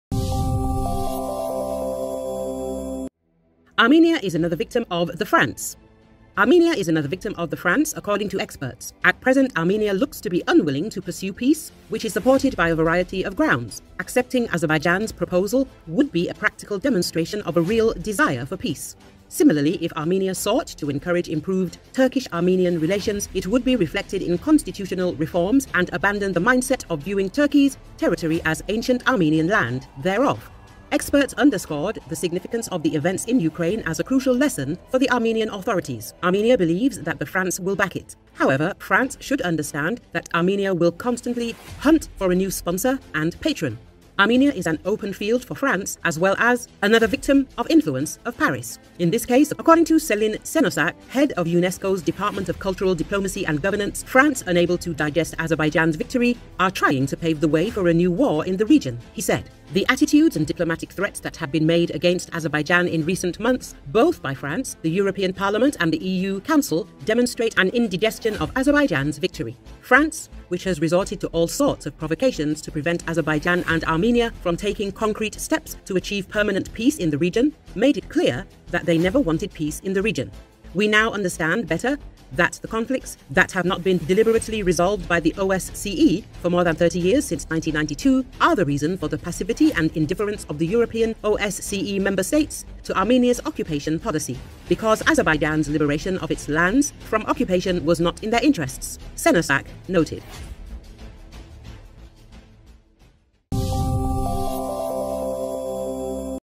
France paving way for new war in South Caucasus region
Armenia is another victim of the France, according to experts. At present Armenia looks to be unwilling to pursue peace, which is supported by a variety of grounds. Accepting Azerbaijan's proposal would be a practical demonstration of a real desire for peace.
Similarly, if Armenia sought to encourage improved Turkish-Armenian relations, it would be reflected in constitutional reforms and abandon the mindset of viewing Türkiye's territory as "ancient Armenian land" thereof.
Experts underscored the significance of the events in Ukraine as a crucial lesson for the Armenian authorities.
Armenia believes that the France will back it. However, France should understand that Armenia will constantly hunt for a new sponsor and patron. Armenia is an open field for France, as well as is another victim of influence of Paris.
İn this case according to Selin Senocak, head of UNESCO's Department of Cultural Diplomacy and Governance, France unable to digest Azerbaijan's victory, are trying to pave the way for a new war in the region, said.
"The attitudes and diplomatic threats that have been made against Azerbaijan in recent months, both by France, the European Parliament and the EU Council, demonstrate an indigestion of Azerbaijan's victory. France, which has resorted to all sorts of provocations to prevent Azerbaijan and Armenia from taking concrete steps to achieve permanent peace in the region, made it clear that they never wanted peace in the region. We now understand better that the conflicts that have not been deliberately resolved by the OSCE for more than 30 years since 1992 are the reason for the passivity and indifference of the European OSCE member states to Armenia's occupation policy. Because Azerbaijan’s liberation of its lands from occupation was not in their interests," Senocak noted.